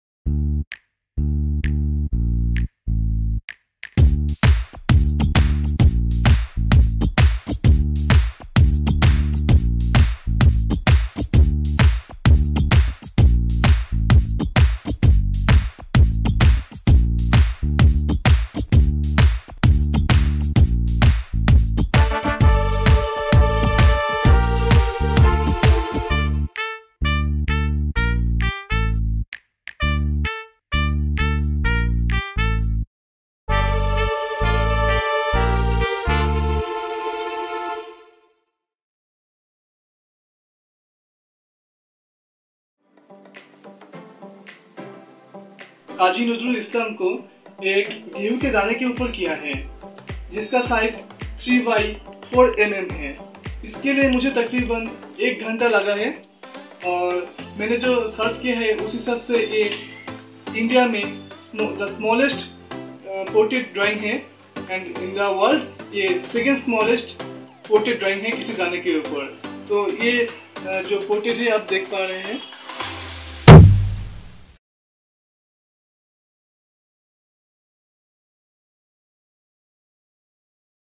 SMALLEST DRAWING OF KAJI NAZRUL ISLAM ON WHEAT PIECE WITH LEAD PENCIL
The World Record of “Smallest Drawing of Kaji Nazrul Islam on wheat Piece with Lead Pencil” Is achieved by Sourav Paul from Dhupguri (West Bengal) India on 14th August 2020.
He drew the smallest sketch of Kaji Nazrul Islam on a piece of wheat of size Width 3 millimeter and length 4 millimeter with Lead Pencil in 1hour